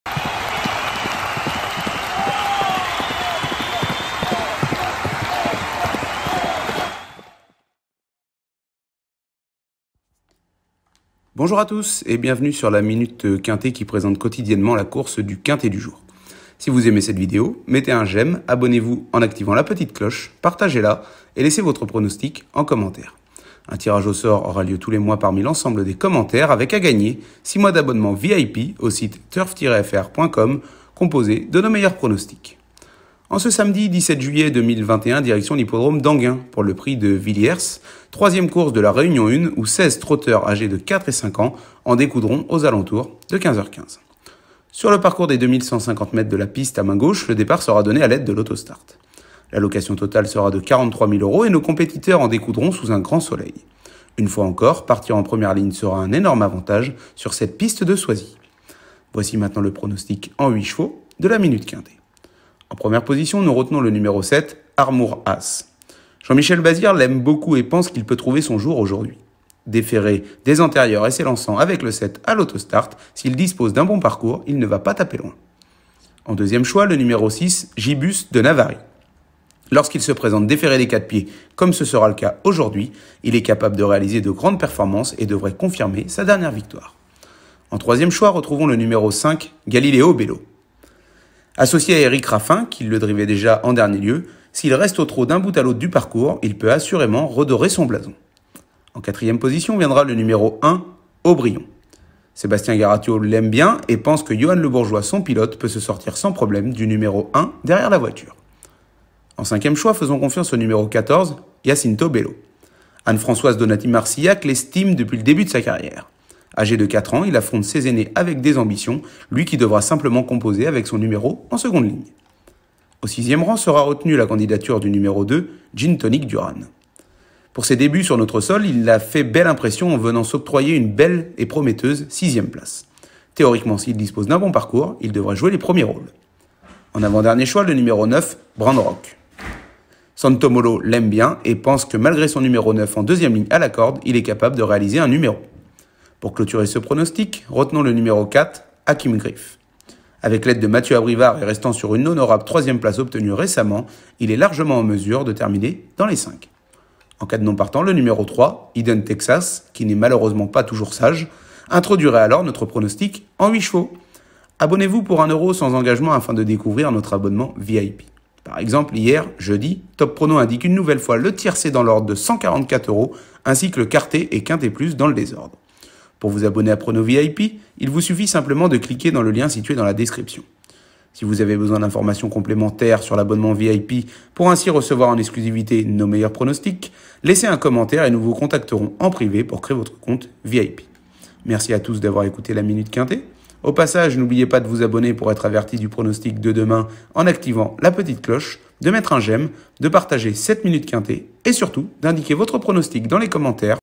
Minute Quinté TURF FR : PRIX DE VILLIERS - Samedi 17 Juillet 2021 - Enghien Soisy 🏇 PMU #246503
🥇 Retrouvez le pronostic exclusif de notre expert Turf Lucius Simenon pour le PRIX DE VILLIERS le Samedi 17 Juillet 2021 à l'hippodrome de Enghien Soisy en France.

✅ NOS LOGICIELS TURF :
✔️ Logiciel TURF INFO Professionnel Edition: